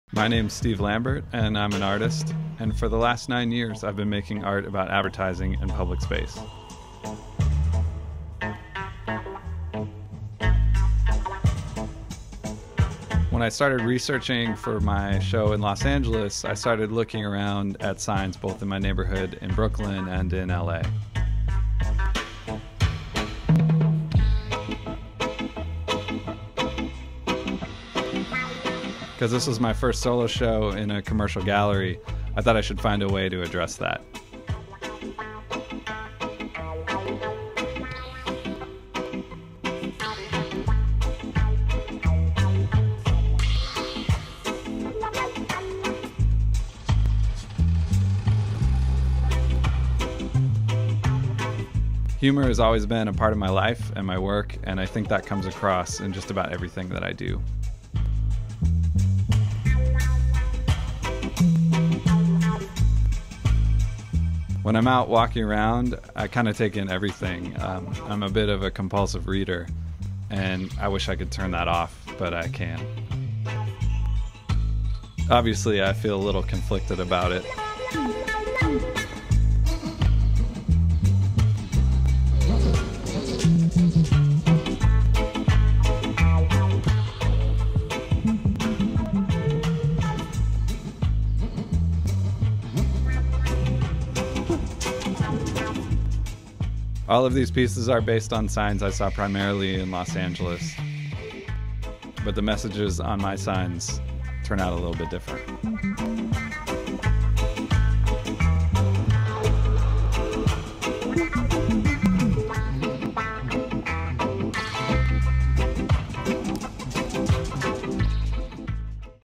Steve Lambert gallery walkthrough
Walkthrough of Steve Lambert's solo show at Charles James Gallery in Los Angeles.  April 25-June 6, 2009.